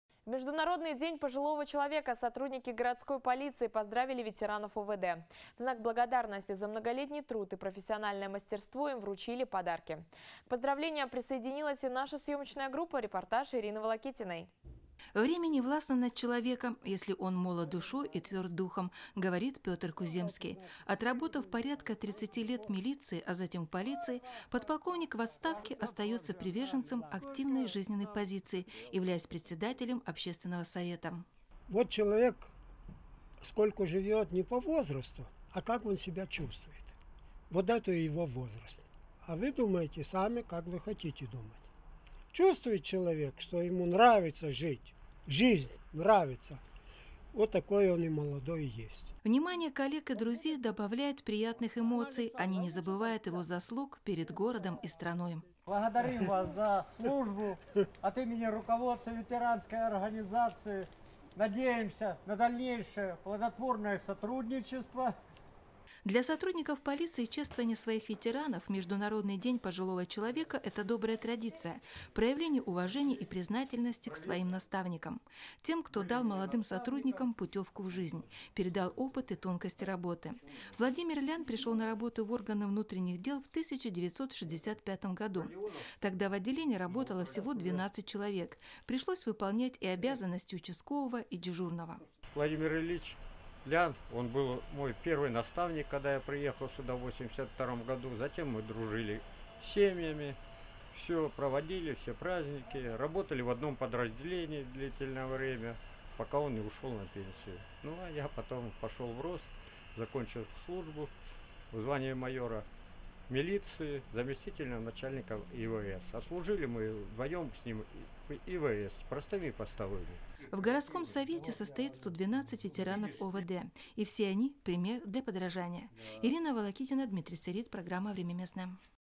Золотой Фонд Лабытнанги
В Международный День пожилого человека сотрудники городской полиции поздравили ветеранов ОВД.